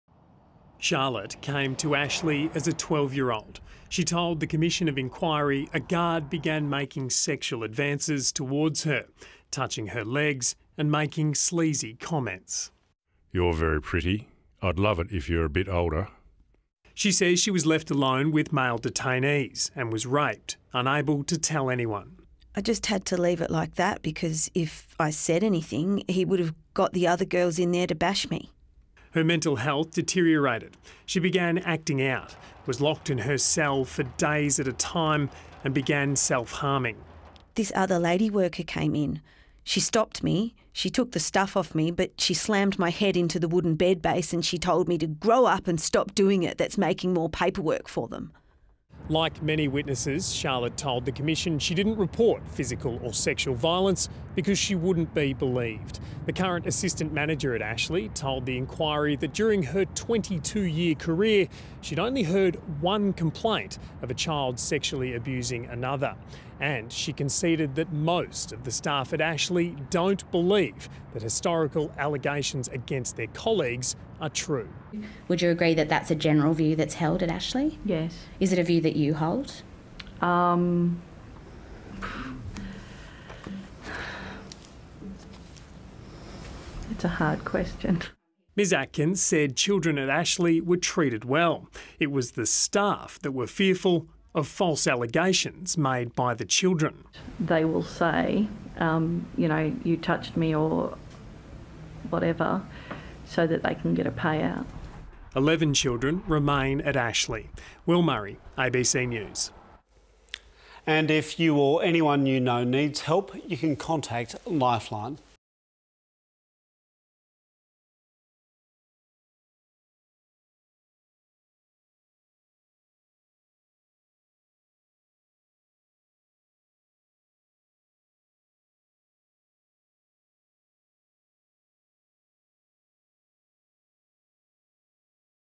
Commission of Inquiry hears of abuse and 'favouritism' inside youth detention centre | ABC News
Warning: Viewers may find details in this video distressing.

Abused, miserable and locked up, youth detainee Charlotte* turned to self-harm — but when she was caught by a youth worker, the response was far from kind.

*Name has been changed.